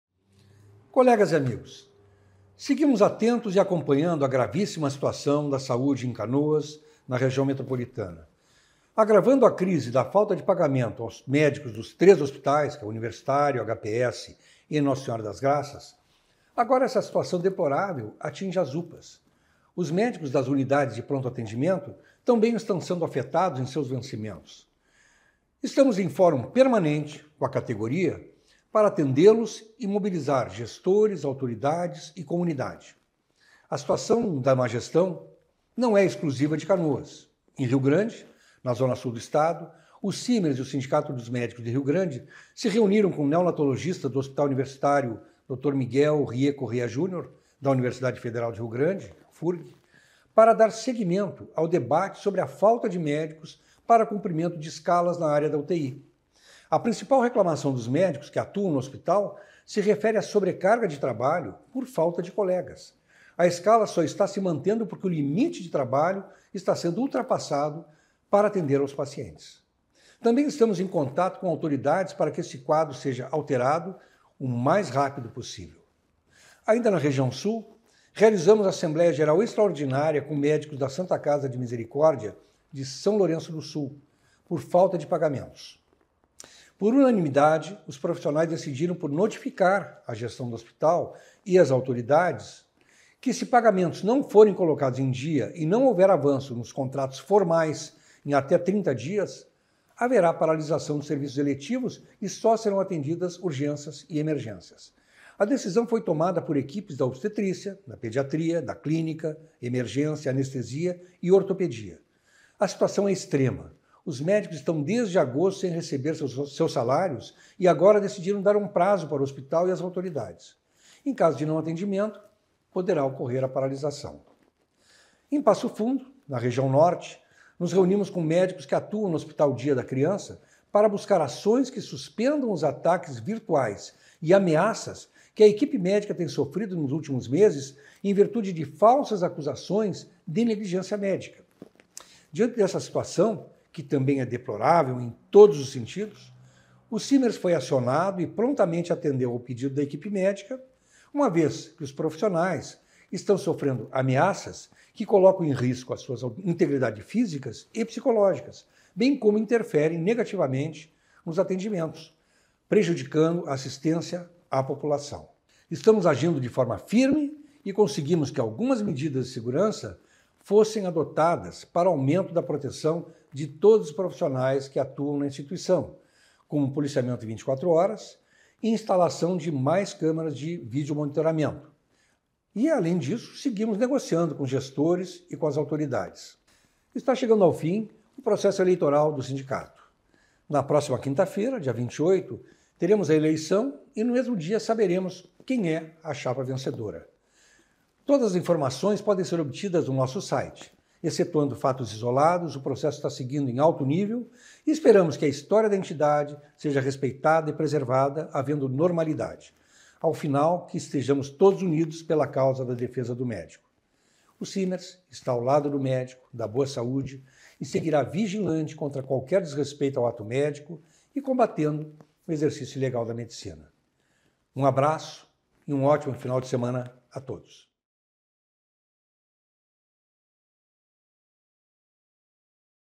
Resumo da Semana - de 18 a 22 de Novembro de 2024
Nesta edição da Semana Simers, o presidente Marcos Rovinski destaca que a entidade segue atenta e acompanhando com apreensão a gravíssima situação da Saúde em Canoas. A situação de má gestão não é exclusiva de cidades da Região Metropolitana. Ele lembra que na Zona Sul do Estado também são encontrados problemas. No Hospital Universitário Dr. Miguel Riet Corrêa Jr., da Universidade Federal do Rio Grande (HU-Furg), diz haver falta de médicos para o cumprimento de escalas na UTI. Em São Lourenço do Sul, há atraso no pagamento dos médicos da Santa Casa de Misericórdia. Por fim, recorda que está chegando ao fim o processo eleitoral do Sindicato, que terá eleição no próximo dia 28. As informações estão disponíveis no site do Simers.